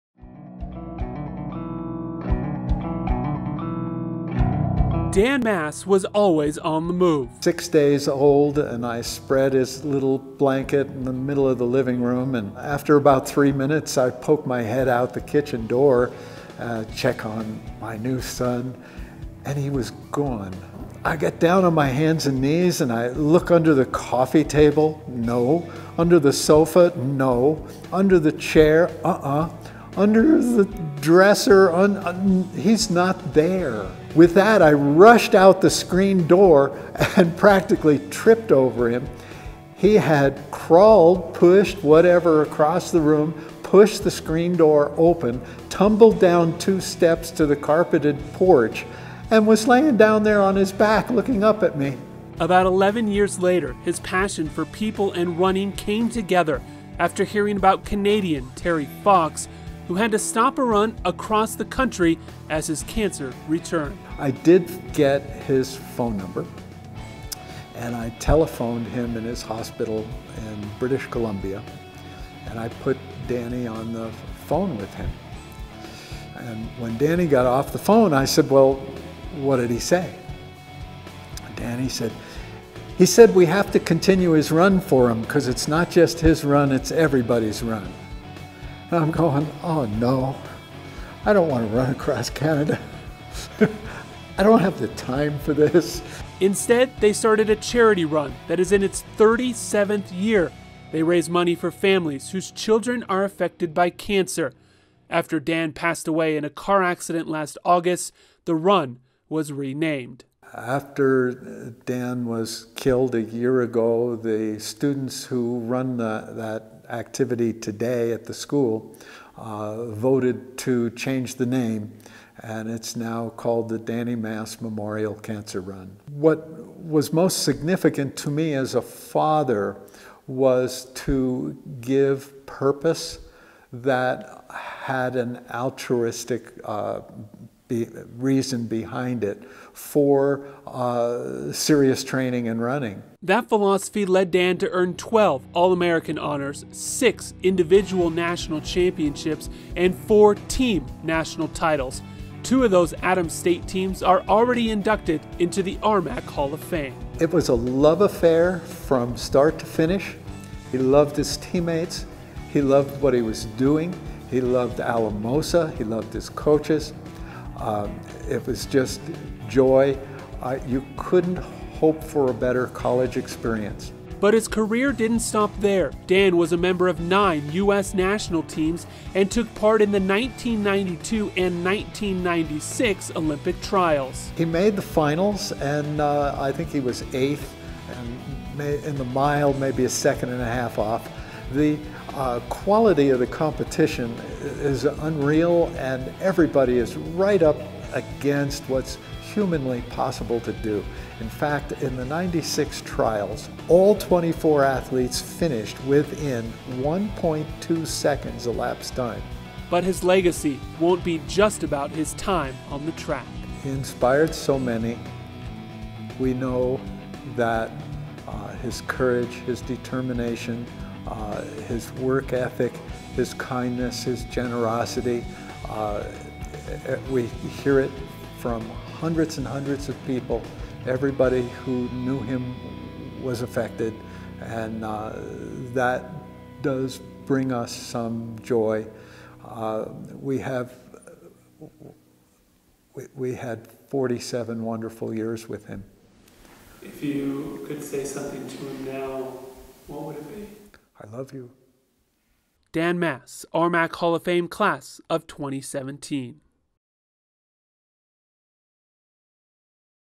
Dan Maas - 2017 RMAC Hall of Fame Feature Story
Maas was a six-time NAIA National Champion during his years at Adams State. He won a title each year, which included the 1989 Outdoor 1500-meter run (3:51.66), 1990 Indoor Mile (4:07.99), 1990 Indoor Distance Medley Relay (10:08.00), 1991 Outdoor 1500-meter run (3:45.31), 1992 Outdoor 1500-meter run (3:43.11), and 1992 Indoor 2-mile run (9:00.10). Maas was also a 12 time All-American and was the leading member of back-to-back NAIA National Championship Cross Country teams in 1988 and 1989. He guided Adams State to its only two NAIA National Indoor Track and Field titles in 1990 and 1992. Maas’ record still stands in the Adams State record books in the 1500-meter run with a time of 3:39.64 and as well as the DMR team that set a record time of 9:42.70. Following his collegiate career, he became a 1992 Olympic Trials Finalist. Maas was inducted into the Adams State Hall of Fame in 2004. He and his teammates from the 1990 and 1992 Indoor Track and Field national championship teams were inducted in to the RMAC Hall of Fame in 2013. Maas passed away on August 2016, at the age of 47.